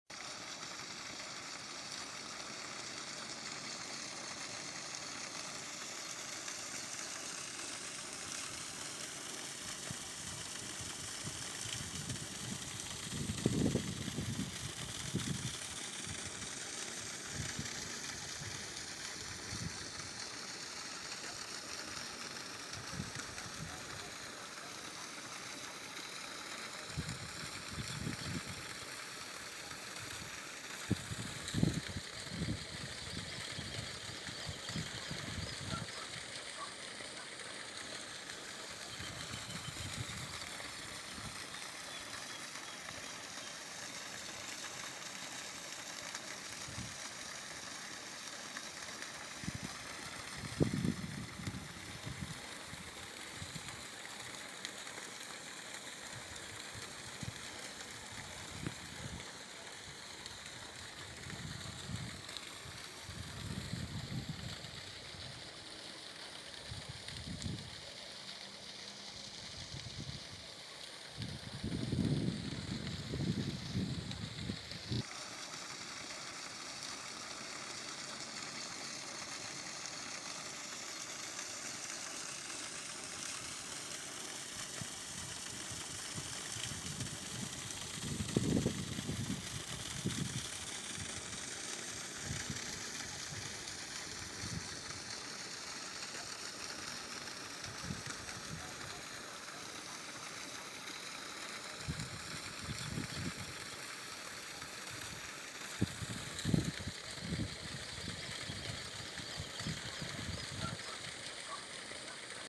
Cloacas a cielo abierto en barrio Los Leños en Puerto General San Martín
Filmado en  Puerto General San Martín
provincia de Santa Fe, Argentina,  1 JUNIO 2020

para NUEVAREGION.COM